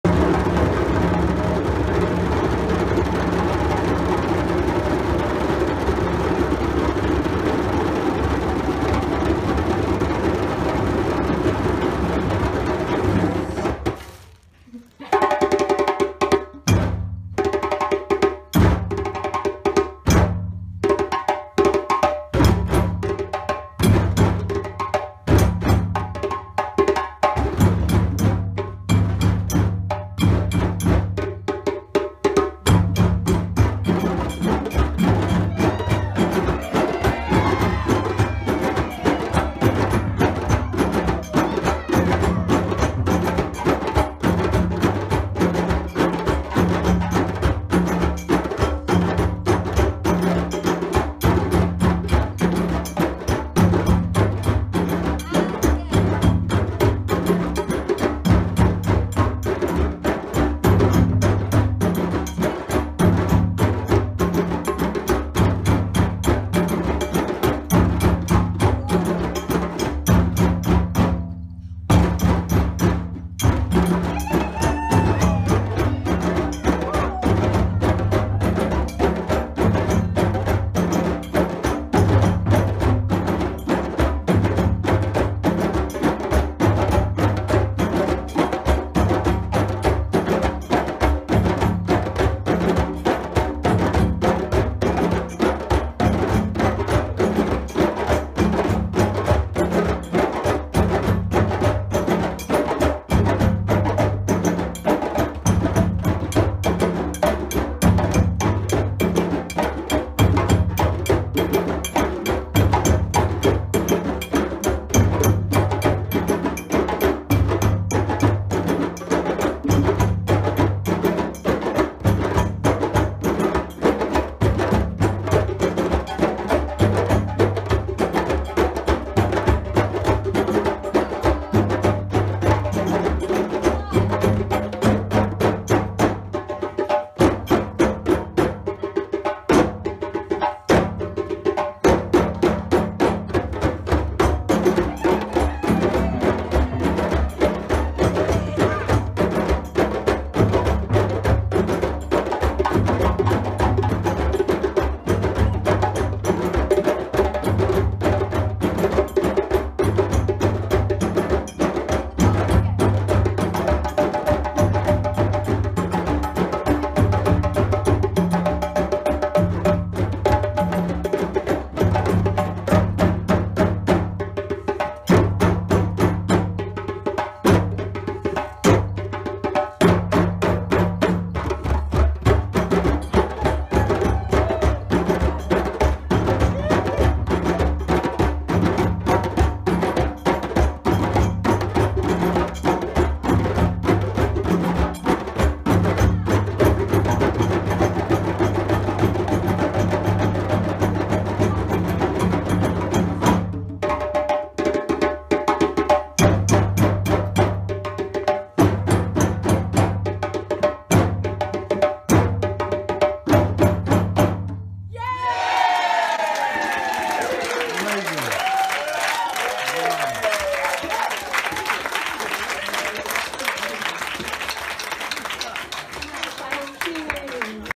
Sarah's workshop @ Kontaani Drumfest 2018
Teaching the wonderful rhythm, Fura, from West Africa, via Hans Sutton.